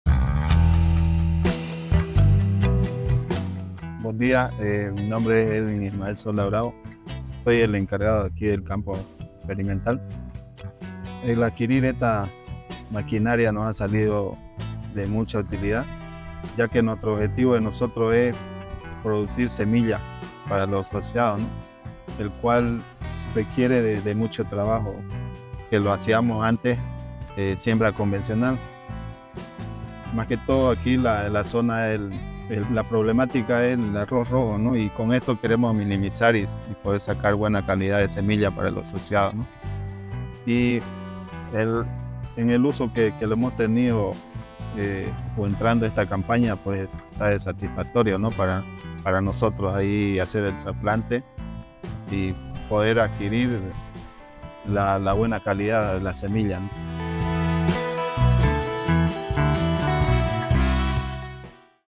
Testimonial Trasplantadora Kubota
"Adquirir esta máquina ha sido de mucha utilidad". 🌾👌
Nuestra Trasplantadora de arroz KUBOTA, es una herramienta práctica que te ahorrará muchos recursos, sin arriesgar la calidad de los resultados. 💯